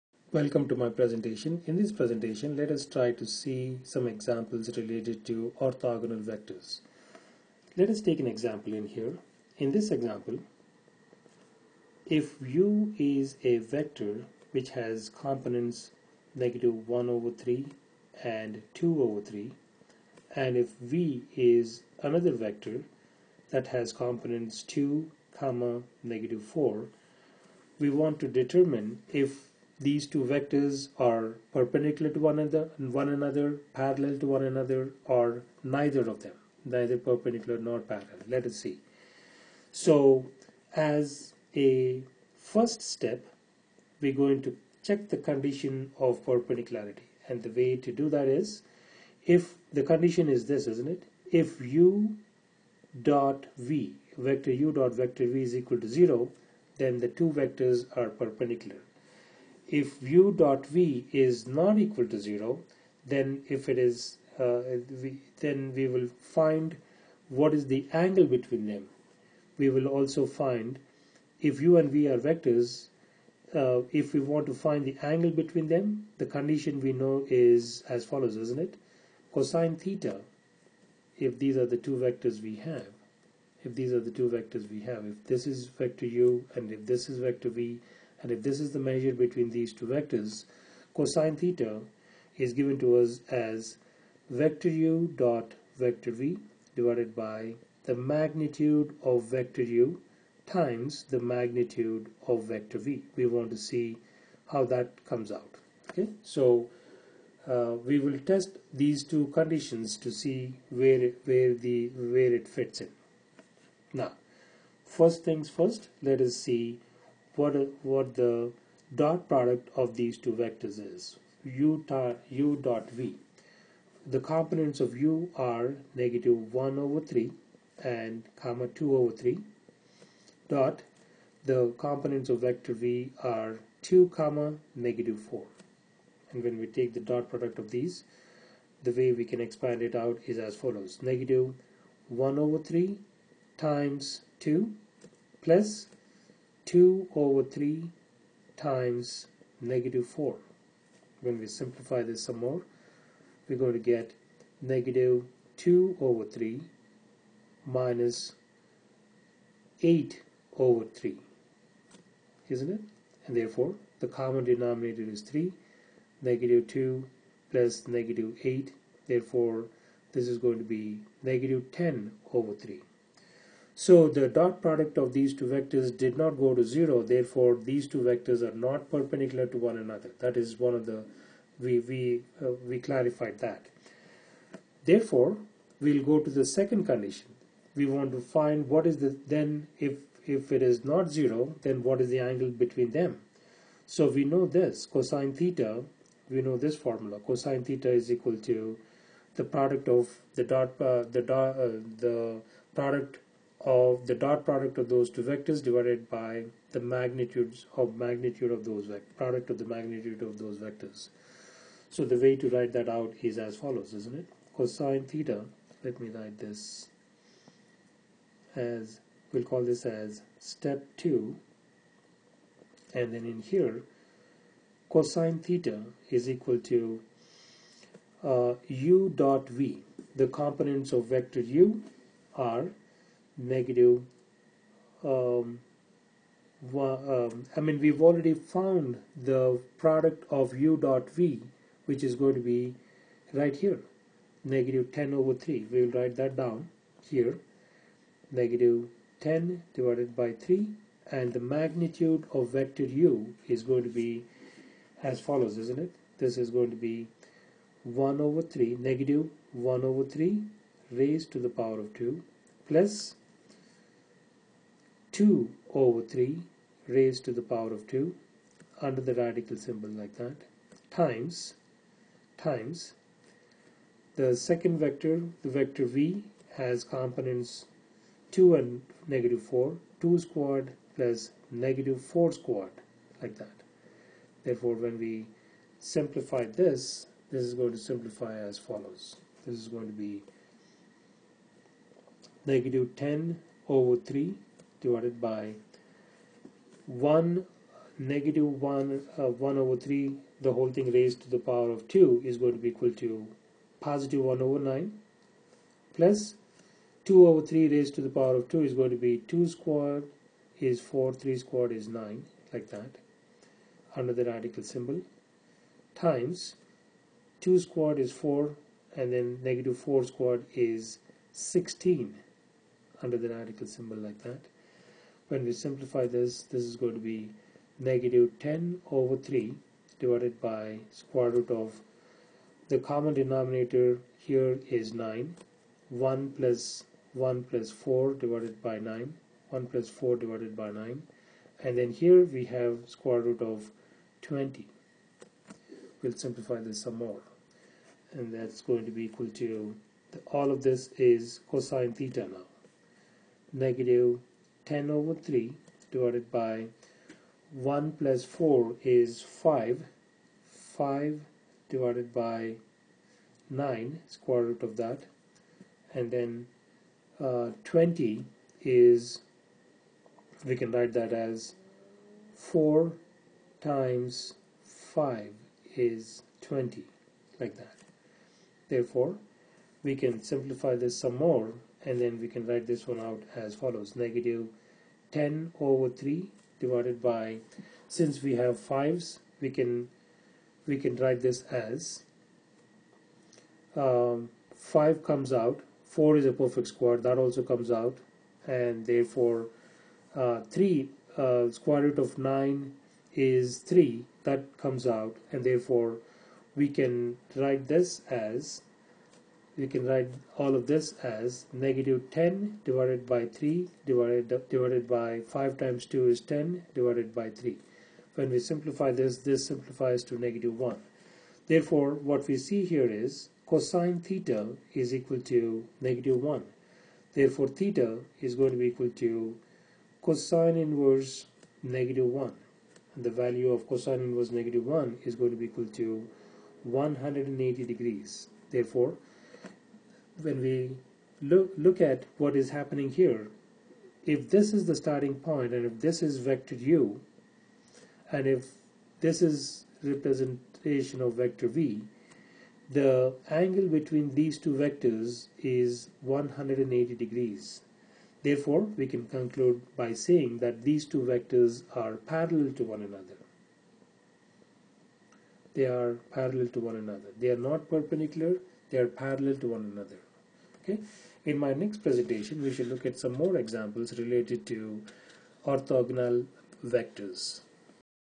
Orthogonal Vectors Example 2
In this presentation we shall how to represent orthogonal vectors with an example.